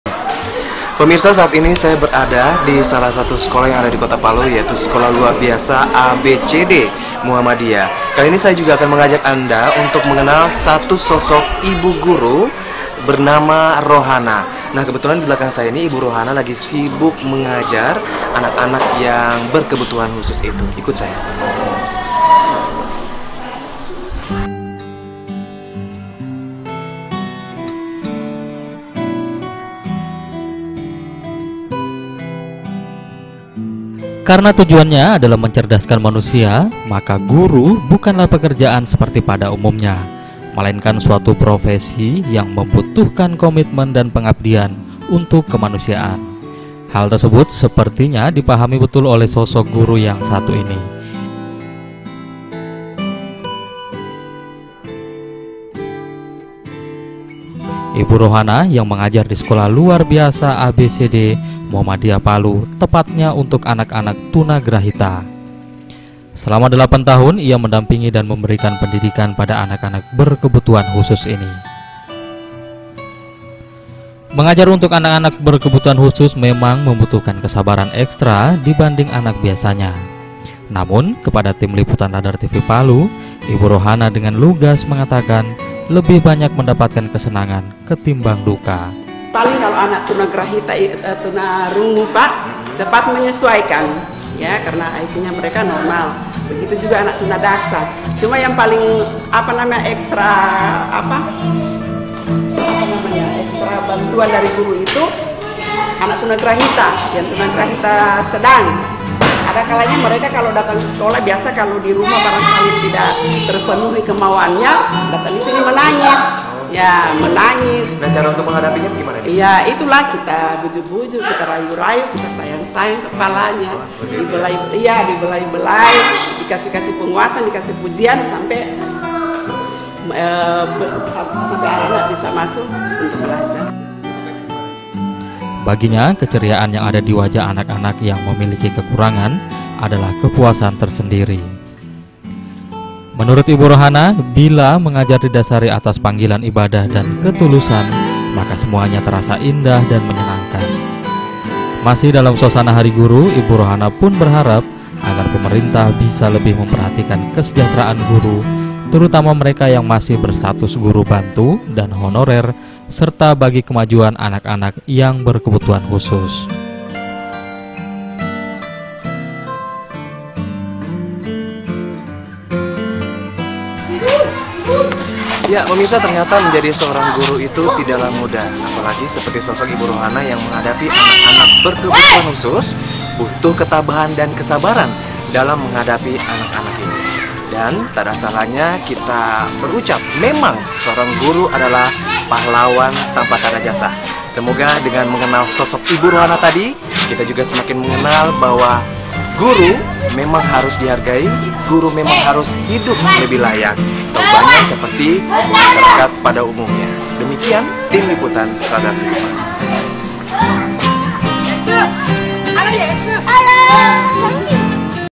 Mengenal Sosok Rohanna, Guru Anak Berkebutuhan Khusus
Guru bukanlah sebuah pekerjaan, profesi ini menuntut pengabdian untuk kemanusiaan. Nah masih dalam suasana Hari Guru Nasional, kali ini tim liputan Radar tv Palu mengajak anda untuk mengenal ibu Rohana; seorang guru bagi anak-anak berkebutuhan khusus yang selama bertahun-tahun setia mendampingi anak-anak didiknya. Ini dia liputannya.